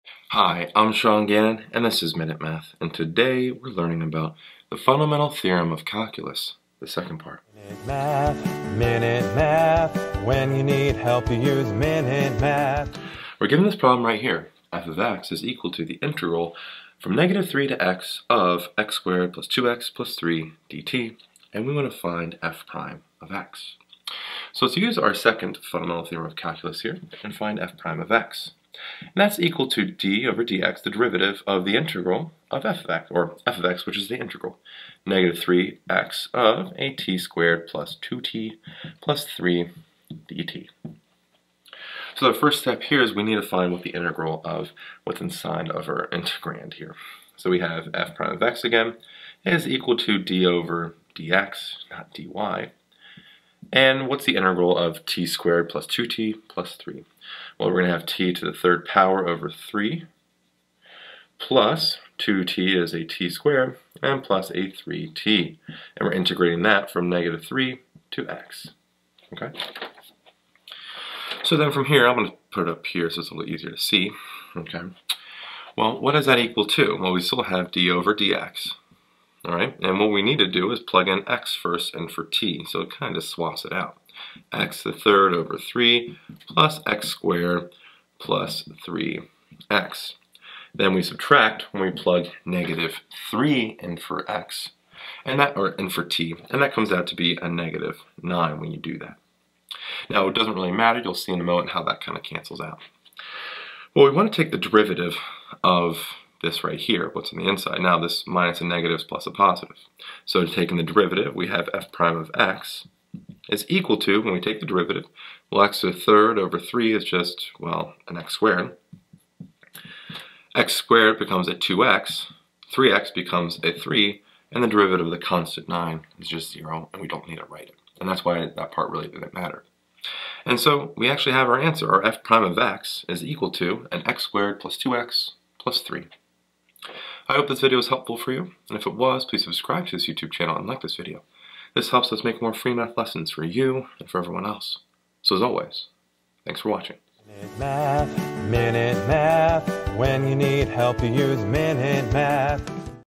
Second Fundamental Theorem of Calculus | Ex. 2 of 8| F(x)=Integral from -3 to x of (t^2+2t+3) dt
The Fundamental Theorem of Calculus (Second Part). Use the Second Fundamental Theorem of Calculus for Integrals to find F’(x) from F(x)=Integral from -3 to x of (t^2+2t+3) dt. We explain how to use the Second Fundamental Theorem of Calculus for Integrals with F(x)=Integral from -3 to x of (t^2+2t+3) dt. We find the integral of the function over the given interval and then find the derivative of that function with respect to x. This is a great Explanation of the Second Fundamental Theorem of Calculus for Integrals. This is under the topic of Definite Integration with this free online math video lesson.

Camera Used For Video (Canon EOS M50):